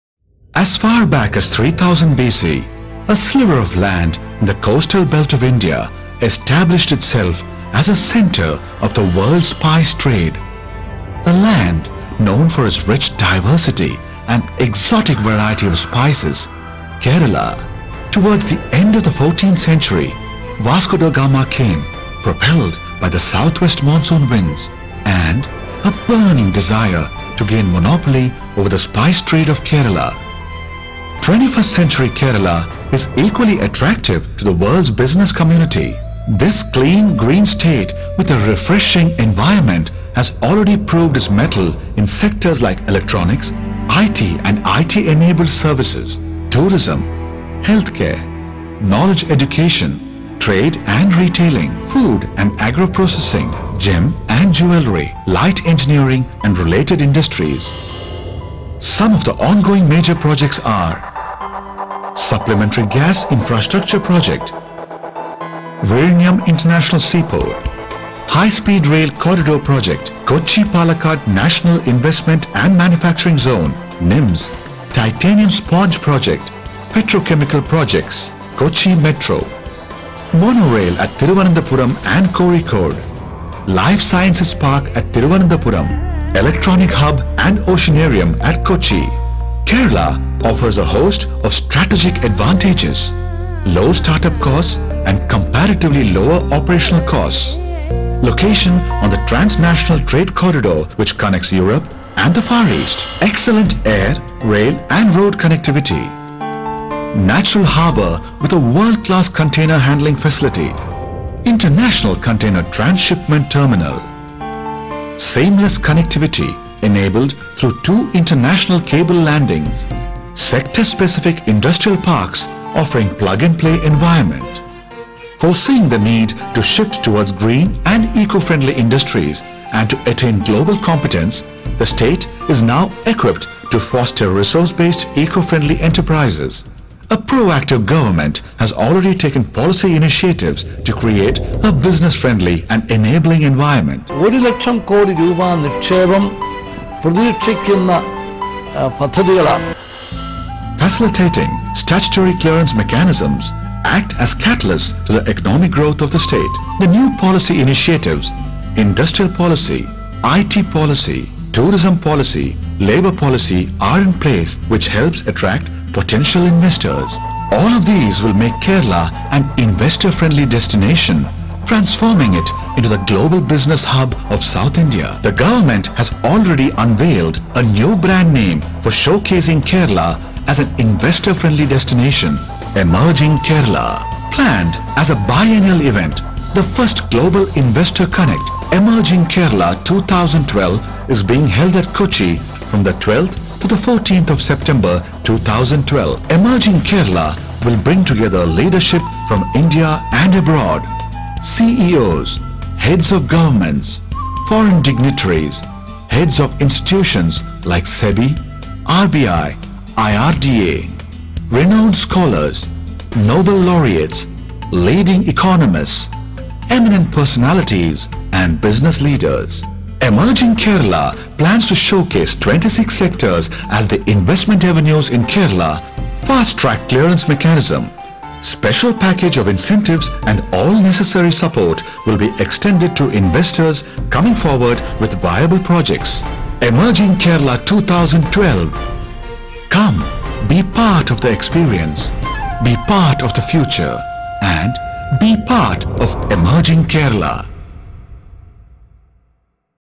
Emerging Kerala - Global Connect
The first edition of Emerging Kerala summit a global connect event will be held from September 12 to 14, 2012 at the Le Meridien International Convention Centre, Kochi. The event will be organised by the Kerala State Industrial Development Corporation (KSIDC) would highlight investment opportunities available in Kerala and advertise to the world its state of readiness to receive investors. It has successfully campaigned itself as God's own country and now became the most sought after destination in the world tourism map.